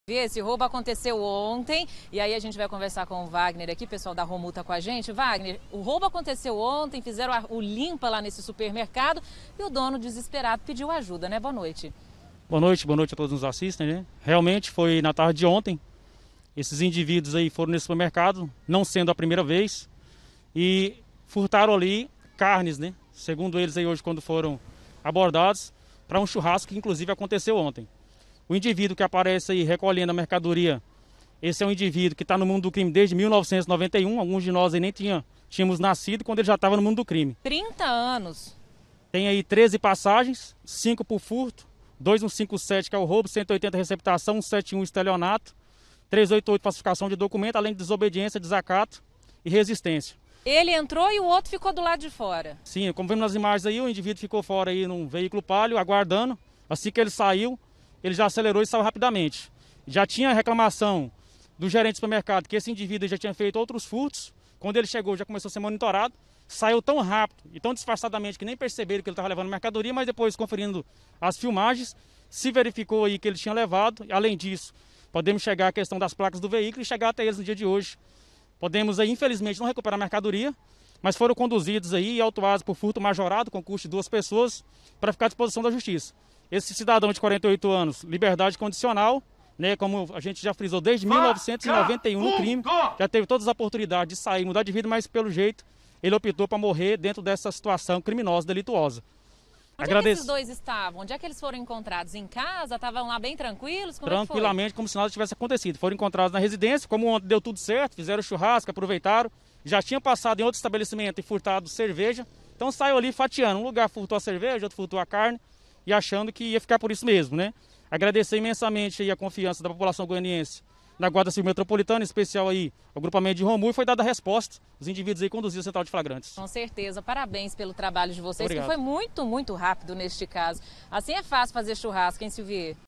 DOIS HOMENS SÃO PRESOS DEPOIS DE FURTAR CARNES EM SUPERMERCADO PARA FAZER CHURRASCO
Dois homens foram presos depois de furtar carnes em um supermercado para fazer um churrasco no final de semana.